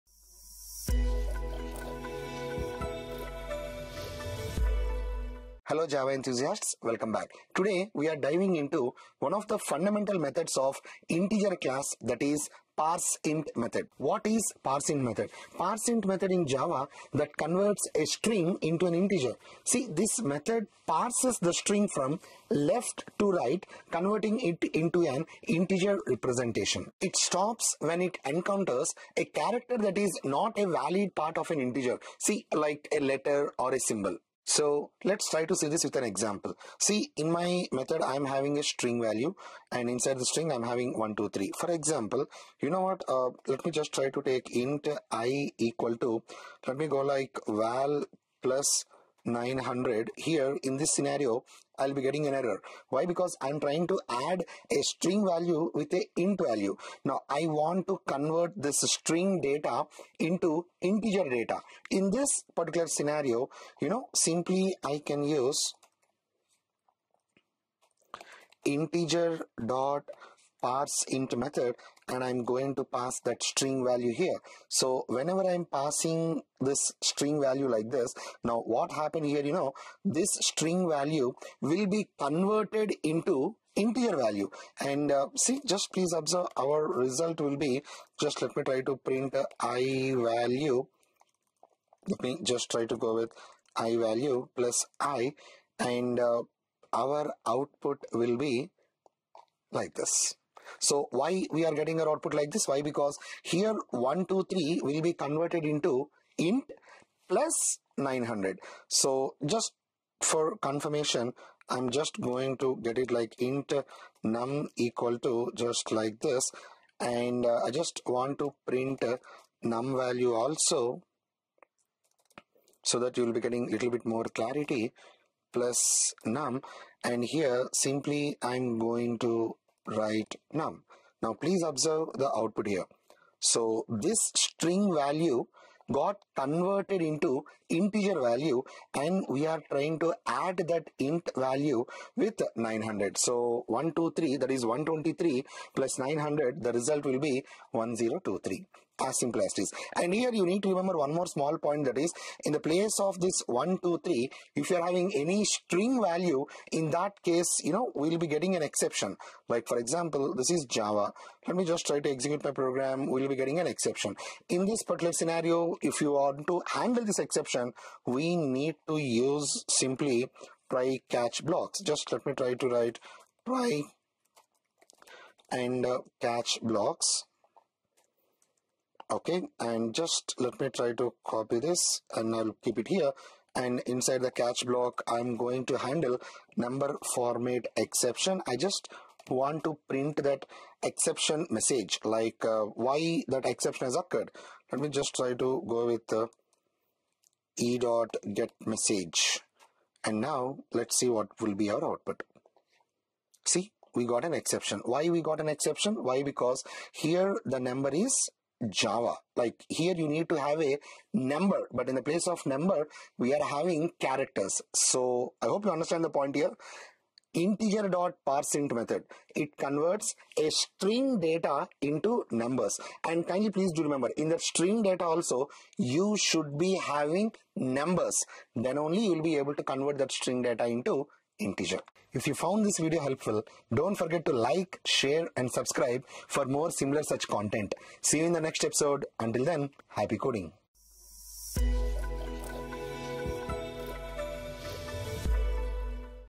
A Guide to Integer.parseInt() for Seamless String to Integer Conversion in Java | NareshIT
👉 Dive into the World of JAVA Developer.

A Guide to Integer.parseInt() for Seamless String to Integer Conversion in Java

Learn how to convert strings to integers in Java using Integer.parseInt(). This video covers basic usage, handling errors, using the radix parameter, and best practices. Perfect for beginners and experienced developers alike!

💡 Our Online Training Features:
• 🎈 Industry-Specific Scenarios
• 🎈 Flexible Timings